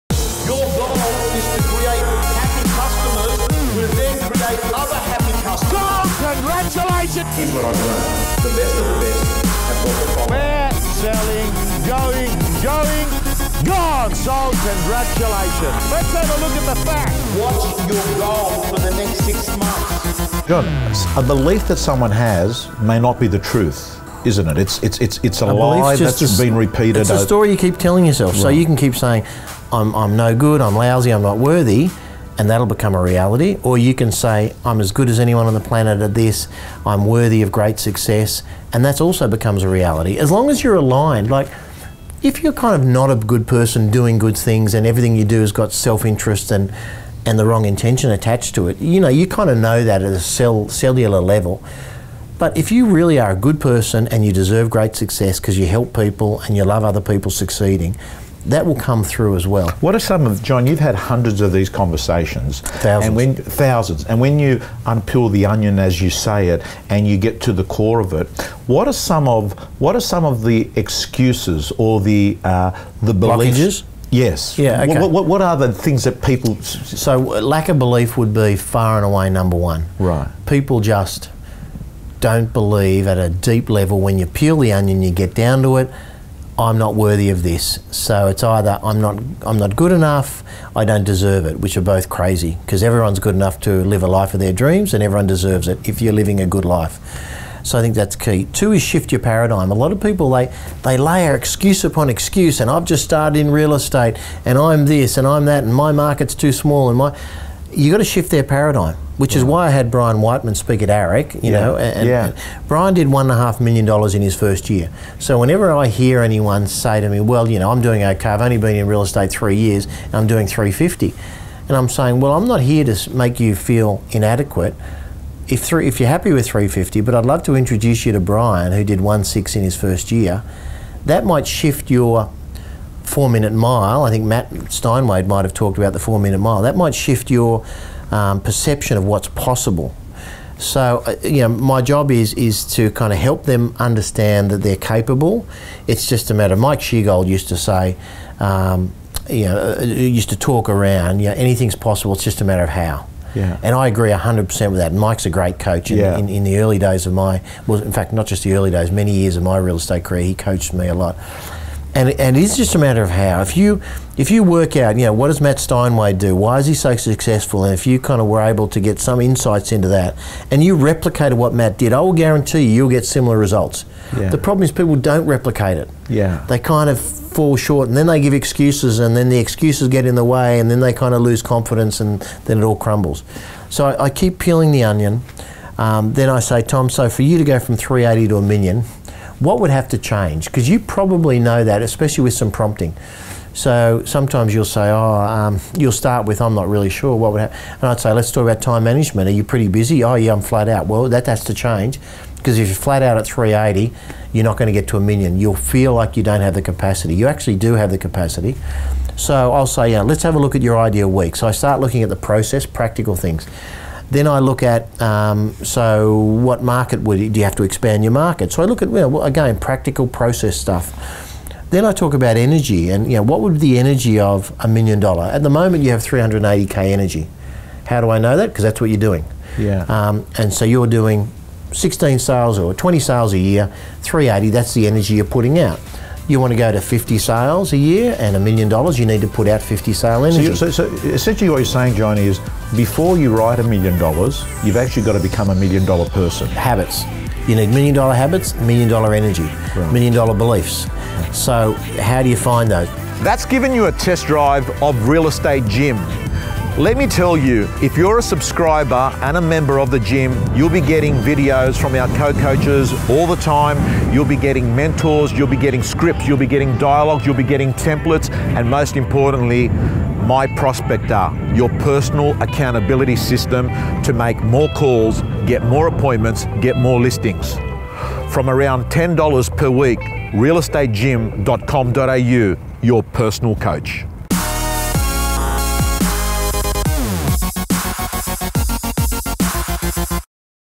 Million Dollar Habits, Energy, Beliefs | John McGrath
This is what it looks like to have a 1:1 Coaching session with John McGrath. Forward this to a colleague and let's 'peel the onion' together. Who you are is not who you can be.